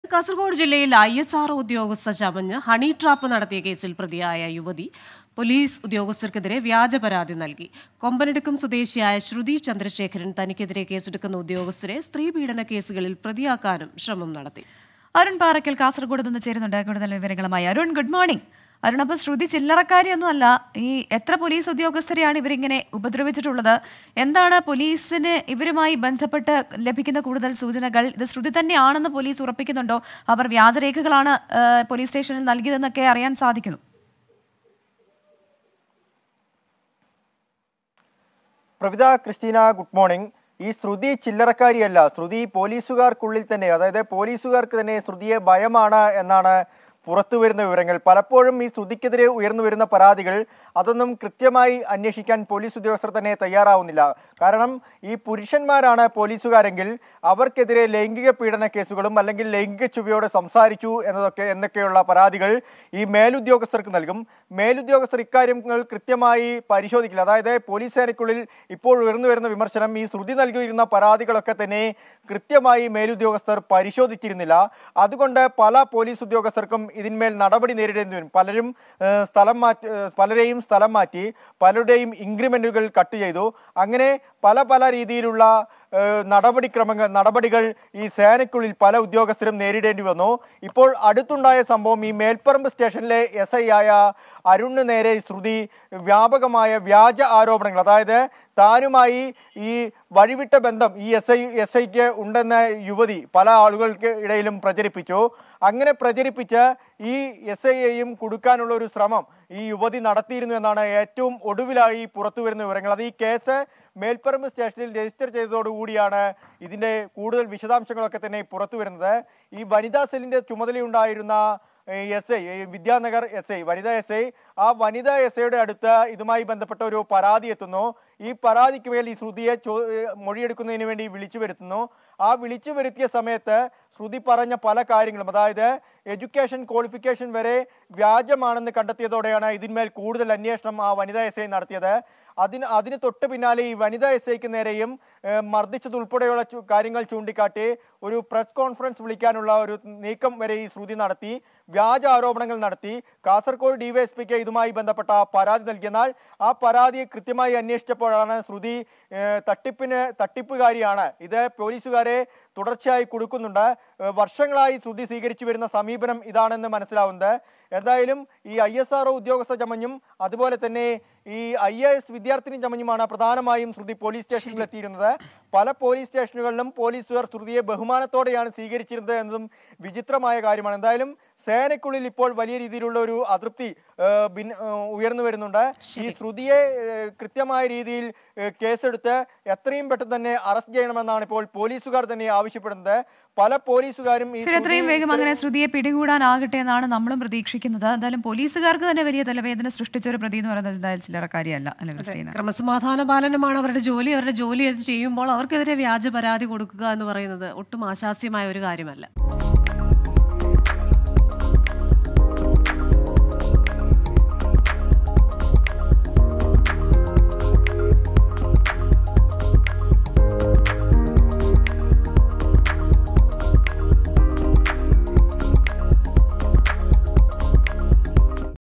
ISRO ഉദ്യോഗസ്ഥ ചമഞ്ഞ് ഹണിട്രാപ്പ്; പിടിയിലായയുവതി പൊലീസ്ഉദ്യോഗസ്ഥർക്കെതിരെ വ്യാജപരാതി നൽകി
ഐഎസ്ആർഒ ഉദ്യോഗസ്ഥ ചമഞ്ഞ് ഹണി ട്രാപ്പ്; പിടിയിലായ യുവതി പൊലീസ് ഉദ്യോഗസ്ഥർക്കെതിരെ വ്യാജ പരാതി നൽകി

accused in honey trap case lodged false complaint against police officers